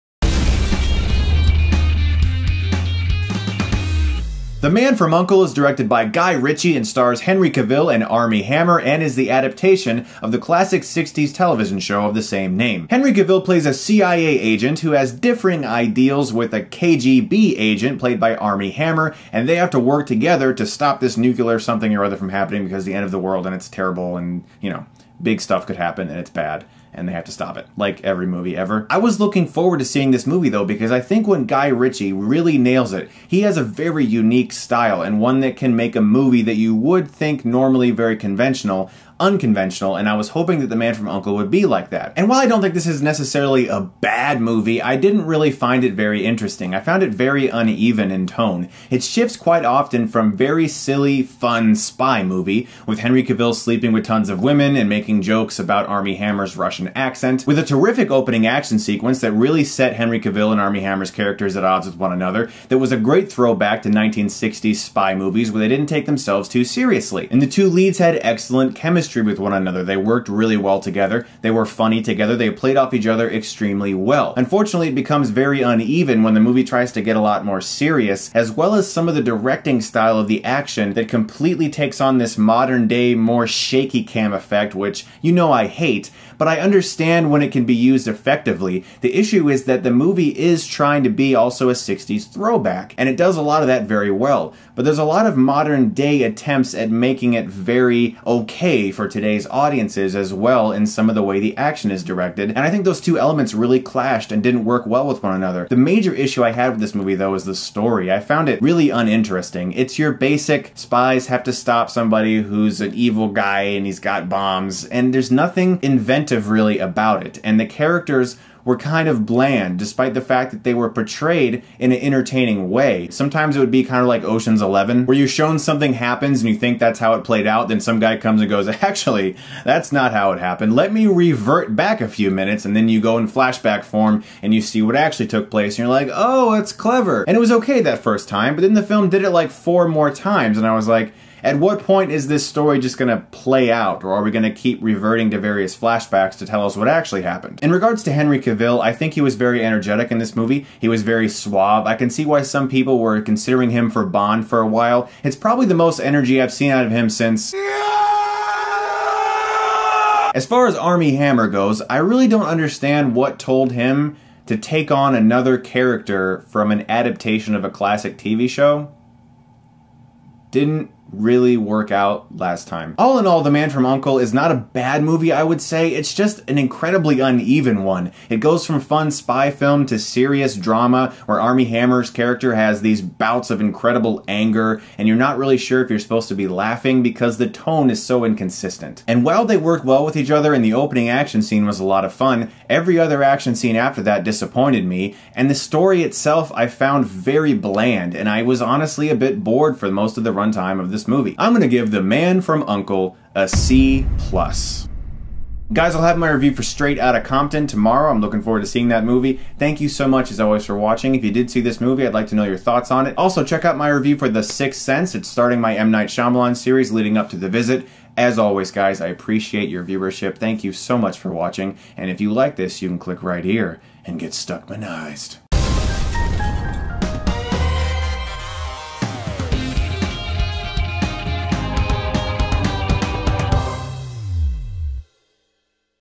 The Man from U.N.C.L.E. - Movie Review Pua
OFFICIAL SITE:

Chris Stuckmann reviews The Man from U.N.C.L.E., starring Henry Cavill, Armie Hammer, Alicia Vikander, Elizabeth Debicki, Jared Harris, Hugh Grant. Directed by Guy Ritchie.